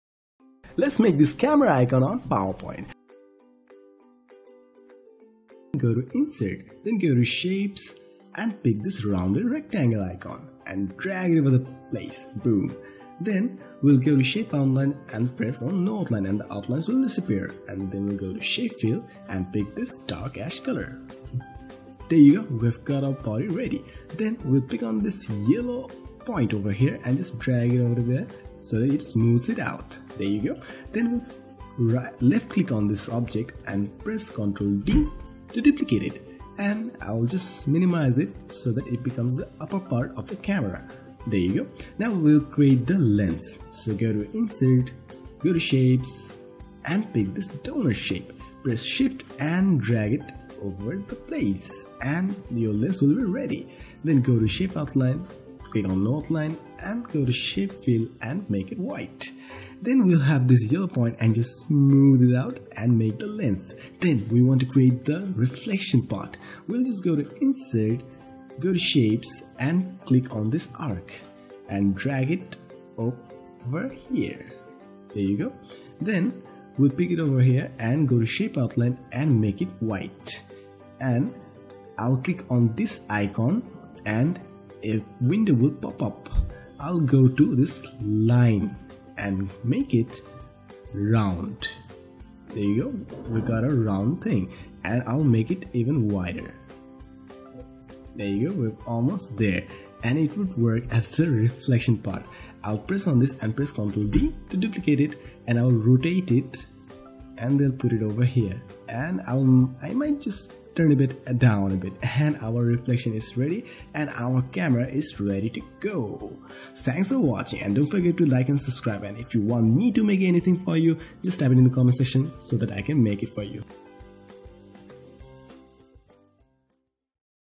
How to Make a Camera Icon On Powerpoint! (Microsoft PowerPoint 2013 Tutorial) | PowerPoint Pro
Make a camera icon with only 3 objects! Follow through this simple video and learn how easy it is to create super cool and simple icons in a blink of the eye.

Powerpoint 2013 was used making this video!

You may suggest any icon you want me to make a tutorial!

You can also check out some of my slide on slideshare!